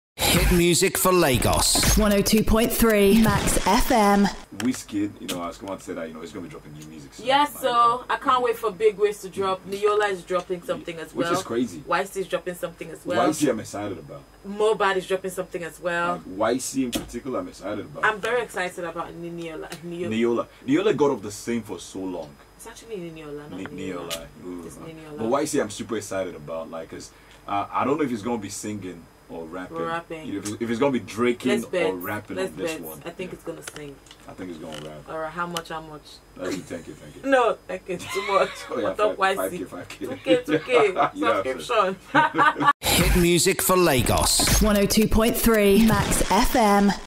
Wizkid, Niyola Set To Drop New Jam And Album [See Video]
Multi-talented singer and songwriter Wizkid has hinted on dropping more hits this summer, the news which got all his fans thrilled and excited.

Grammy Award-winning Wizkid took to his Instagram stories to announce a new song and also a possible album name change, although he didn't specify the date of this new jam, but he speculated around summer, “New music soon come and loads of it summer bout to be crazy,” he wrote on his Instagram stories.

MaxFm 102.3 3rd May 2022